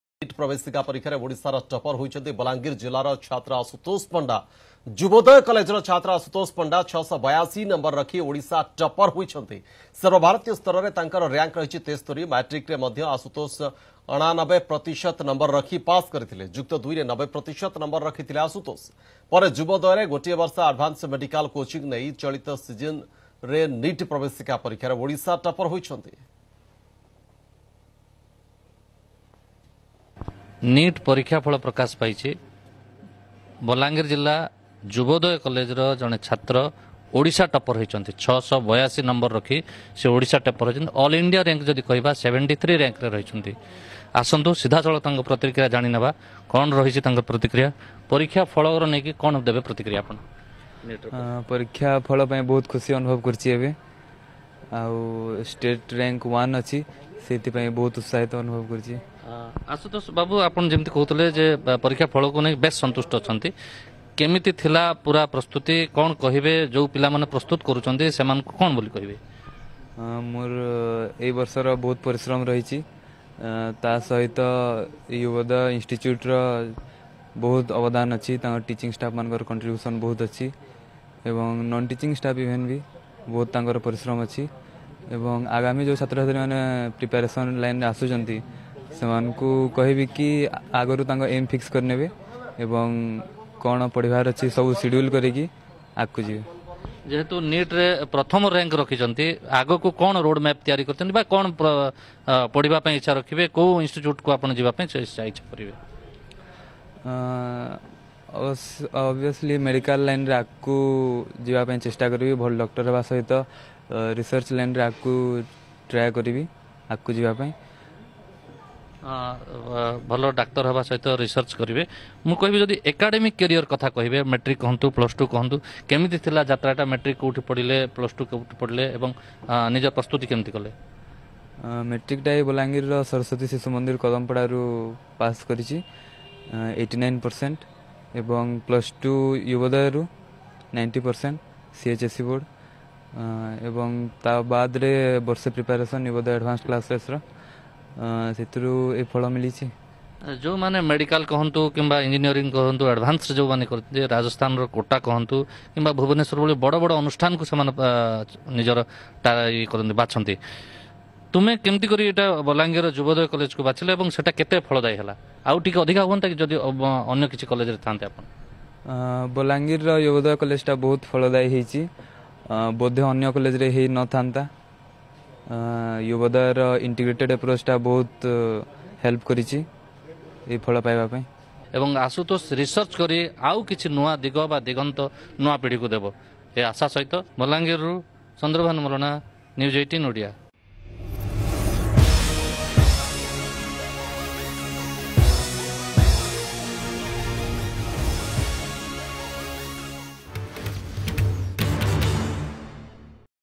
NEET-2019 Odisha Topper
Asutosh Panda student of Yuvodaya Classes,Bhubaneswar got 682 marks on NEET medical entrance Odisha Topper